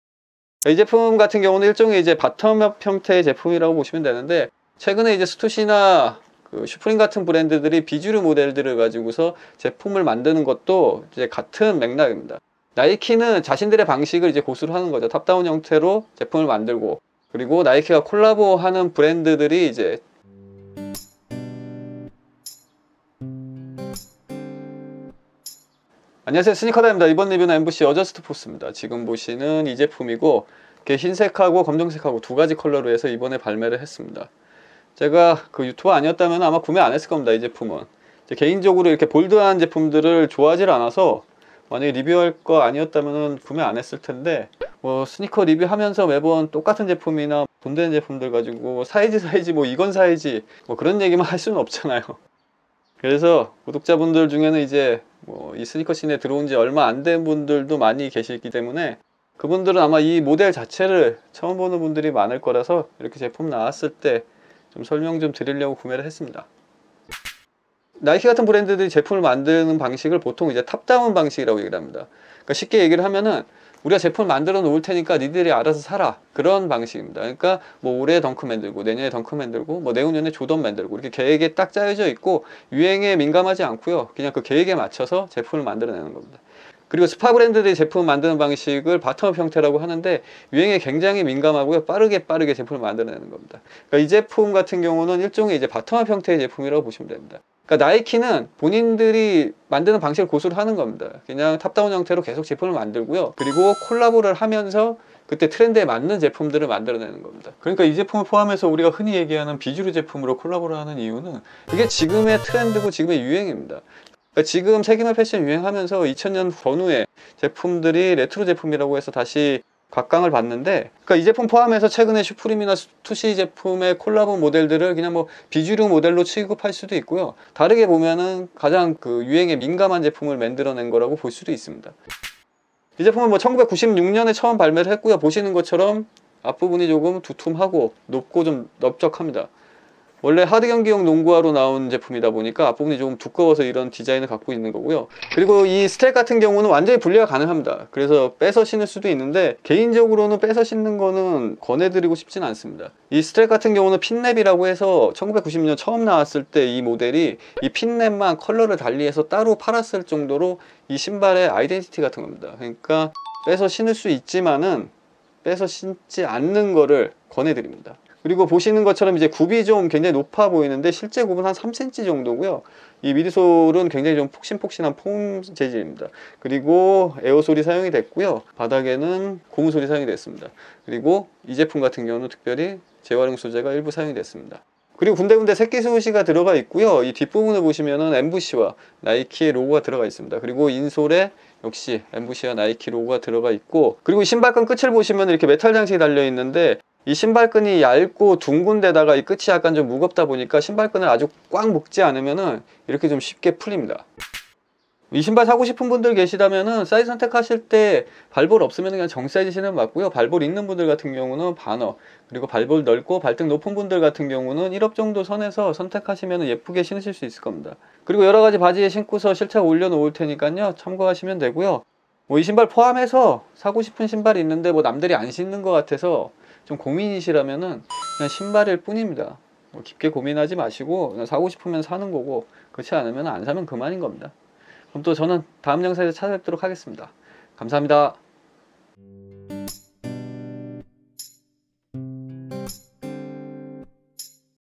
최근 나이키 콜라보에 비주류 모델이 부쩍 많아진 이유 (요즘 신발 뭐삼? #56)
요즘 신발 뭐삼?
Nike x Ambush Air Adjust Force SP Summit White and Black

나이키 x 앰부쉬 에어 어저스트 포스 SP 서밋 화이트 앤 블랙

DM8465-100

it's AUTHENTIC !